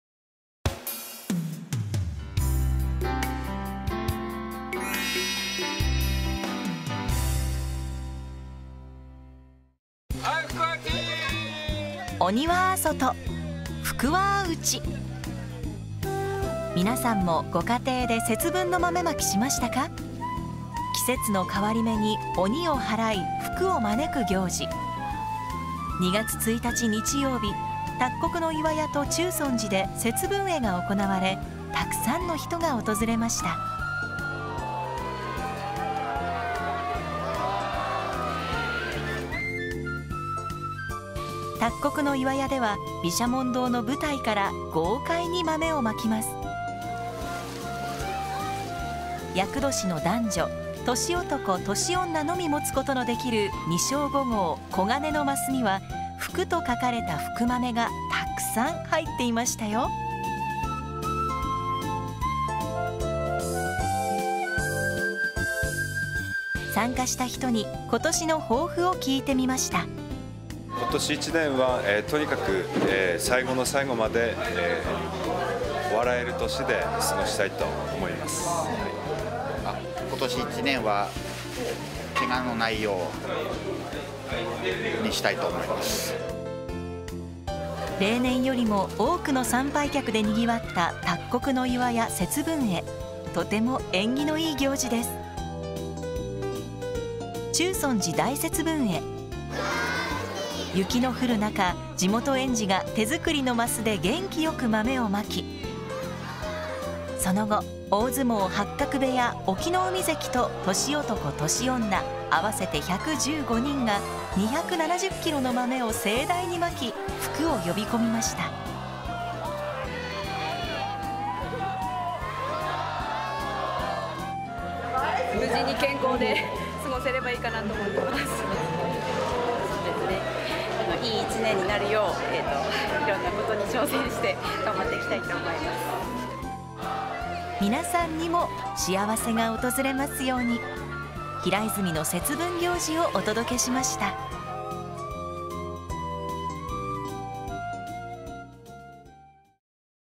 【English】All About Hiraizumi#38 Setsubun events for inviting good luck for the year
Broadcasted on February 11th, 2015

　Evil out!
　Good luck in!
　Did you hold ‘Bean throwing on Setsubun’ at home?
This is the even to ward off evils and invite good luck at the change of season.
On Sunday, February 1st 2015, Setsubun-e events were held at Takkoku no Iwaya and Chuson-ji, and many people visited there to join the events.

　At Takkoku no Iwaya, people throw beans with all their strength from the stage of Bishamon Hall.
Only men and women in yakudoshi (unlucky year) and toshi-otoko and toshi-onna (men and women of the same Chinese zodiac animal as the current year) are allowed to have 2 sho and a half golden square cup, containing a lot of ‘fuku-mame’ on which a Chinese character ‘fuku (happiness) is written.

　We interviewed some participants about their resolution for this year.
　　I would like to spend this year with a smile until the very end.

　　Throughout this year, I would like to be careful not to hurt myself.

　Setsubun-e at Takkoku no Iwaya is very auspicious event and it was very crowded with more visitors than usual this year.

　Dai Setsubun-e (grand Setsubun-e) in Chuson-ji
Although it was snowing, children from local kindergartens cheerfully scattered beans from their hand-made cups, then 115 people in total including the sumo wrestler, Oki no Umi from Hakkaku stable and toshi-otoko and toshi onna (men and women of the same Chinese zodiac animal as the current year) threw 270 kilograms of beans splendidly to invite good luck.

　Participants’ resolution for this year
　　I hope we can stay healthy without getting injured for this year.

　　To make this year satisfying, I will challenge and do my best in various things.

　We wish you a happy year…
We introduced Setsubun events in Hiraizumi Town to you.

Tourism and Commerce Division, Hiraizumi Town Hall